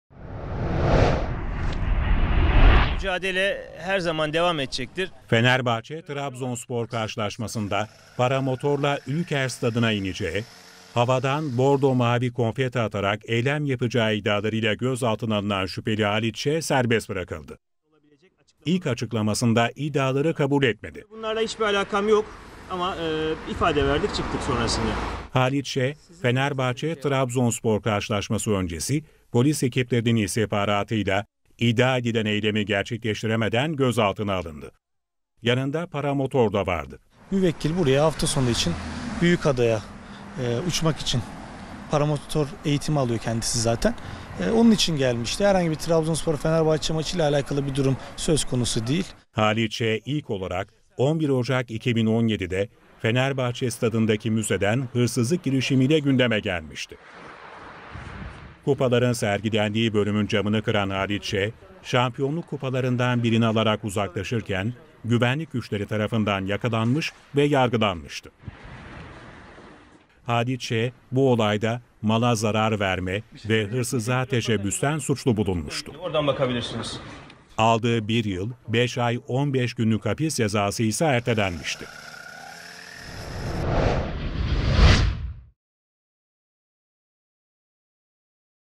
Fenerbahçe-Trabzonspor maçına kendi hava aracıyla konfeti saçmak istedi
2 yıl önce Fenerbahçe stadından şampiyonluk kupası çalmak isterken yakalanmıştı. Şimdi de Fenerbahçe-Trabzonspor karşılaşmasında paramatorla stada gidip havadan bordo mavi konfeti atacağı iddiasıyla gözaltına alındı. Serbest bırakılan şüpheli iddiaları kabul etmedi.